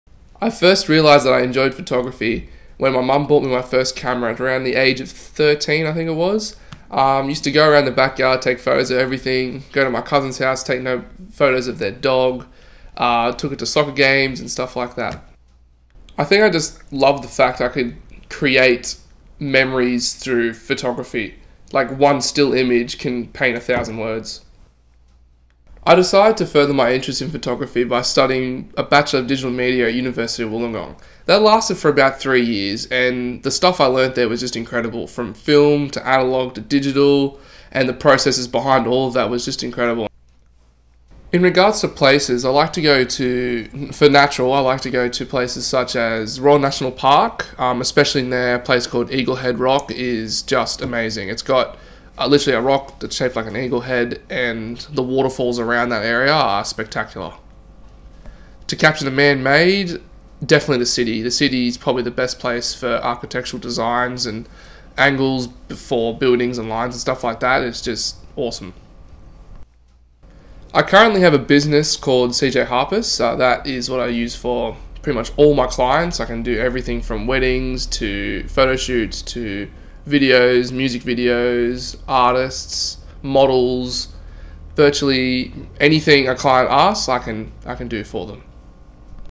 AustenPeck JRNL102 AudioVisual
CJ Harpas talking about his passion and how he discovered photography and where it's taken him.